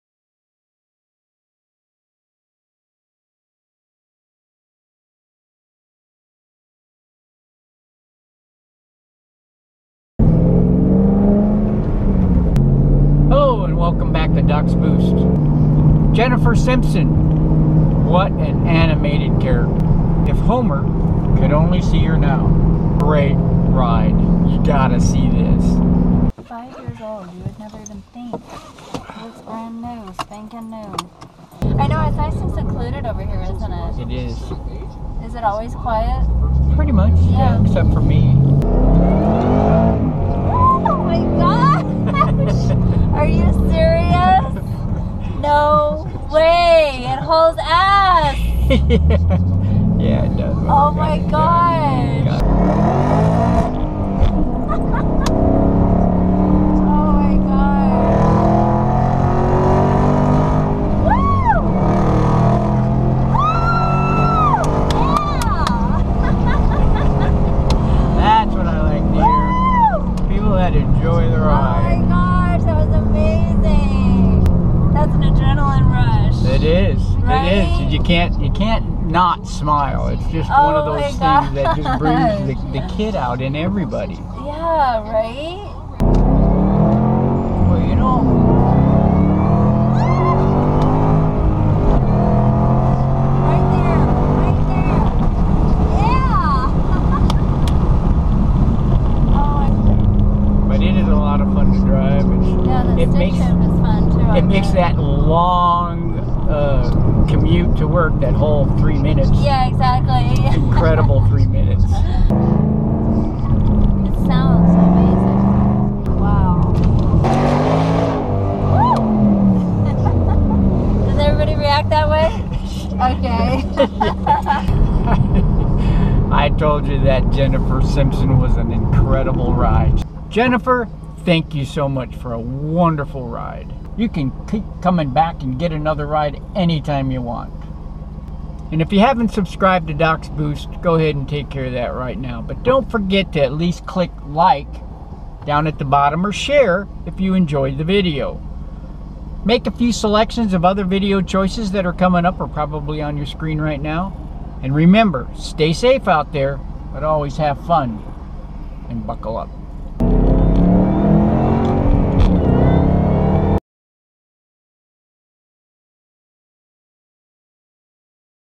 Jennifer Simpson Rockin It with Doc's GT500
This is what is all about.  Unbridled excitement.  Sometimes you cannot tell how thrilling power can be, others, for example Jennifer broadcasts it to the world.  When a person does that, it affects everyone around, and everyone who watches. Enjoy and click Like.